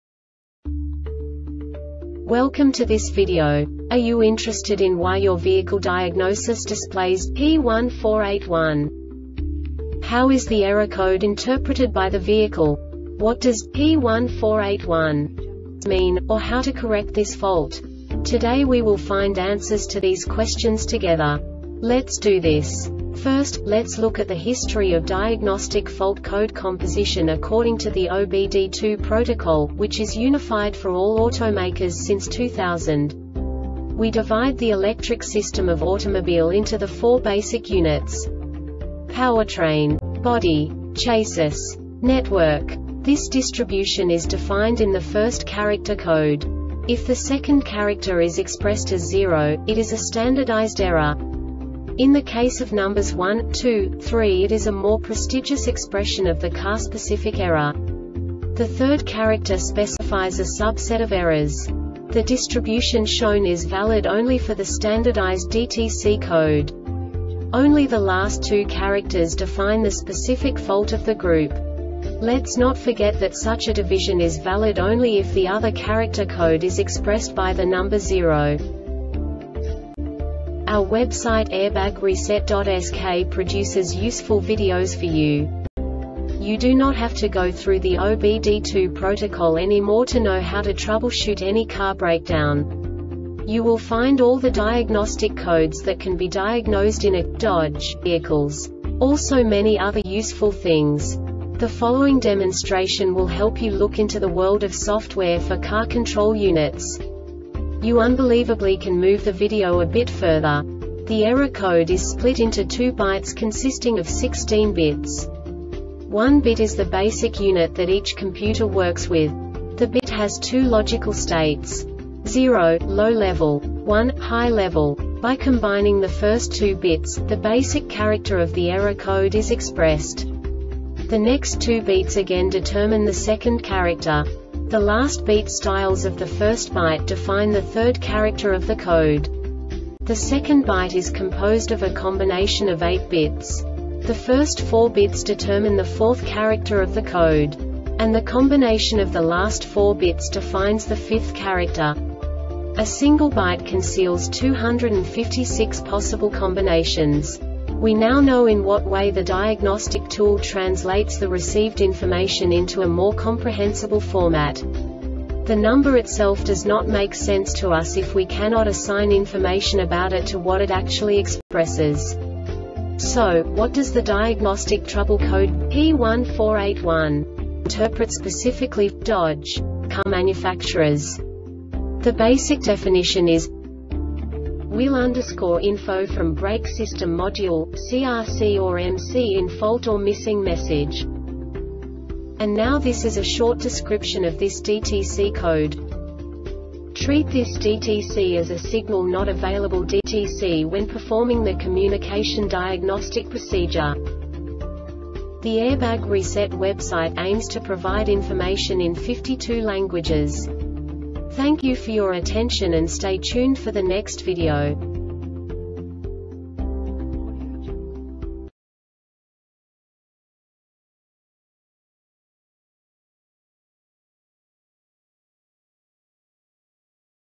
DTC Dodge P1481 Short Explanation
Contents:
0:21 Basic DTC analysis according to OBD2 protocol standard.
1:48 Insight into programming
2:58 A diagnostically understandable description of the Diagnostic Trouble Code
3:05 Basic error definition

Make:
Dodge
Code:
P1481
Definition:
Wheel_Info (From Brake System Module) - CRC or MC In Fault Or Missing Message
Description:
Treat this DTC as a SIGNAL NOT AVAILABLE DTC when performing the COMMUNICATION DIAGNOSTIC PROCEDURE.